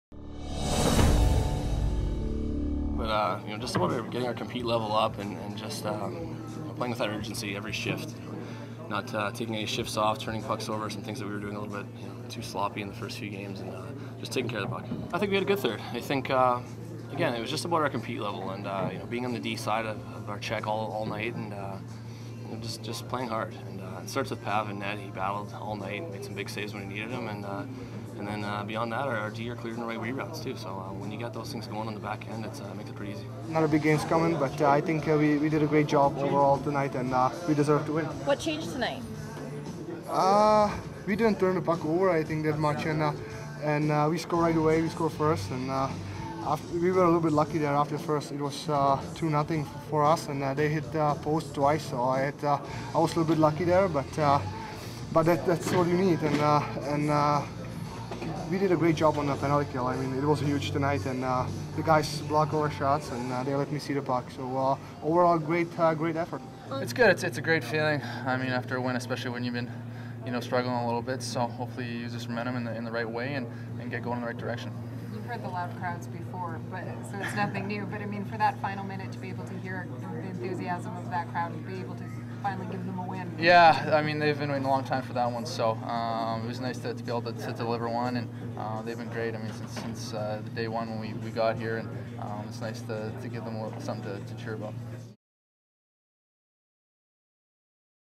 Jets players talk after October 17 win
October 17, 2011- Ladd, Pavelec and Glass talk after the Jets first win of the regular season, 2-1 over the Penguins.